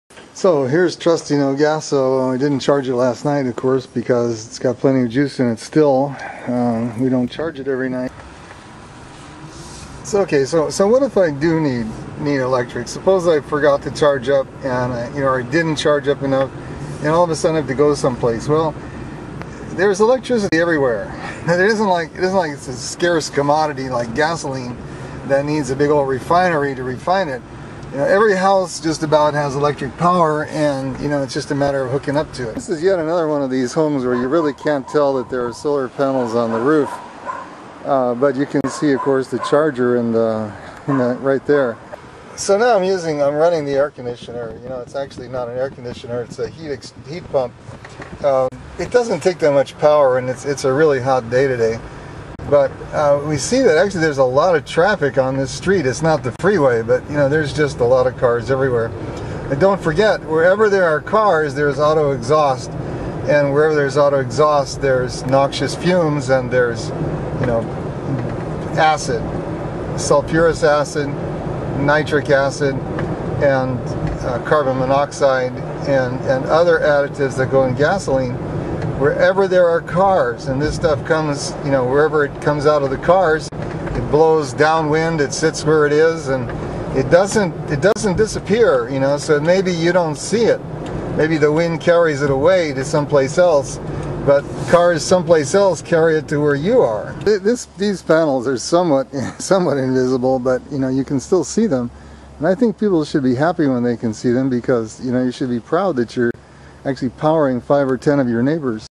Oct. 2 Solar Tour EB Gendel and Guoz LB homes - tape 1
Electric is everywhere.  An EV can charge up at almost any home, unlike gas cars that require a specialized infrastructure and huge refineries to make the fuel.

It's gas cars that are difficult to refuel, not EVs.

The Long Beach home of EB Gendel, who has a plug-in all-electric RAV4-EV and charges if from solar PV credits.

Even beautiful residential areas have plenty of cars, and plenty of SOx and NOx -- nitrous and nitric acid, sulfurous and sulfuric acid when oxidized by sunlight in the presence of Ozone.  Even if it blows away, there's other cars and refineries that are upwind of each area.

Guoz solar home in Long Beach does its part to lower daytime peak and avoid the need for new nuke and dirty peaker power plants.  This is the most valuable, daytime electric power.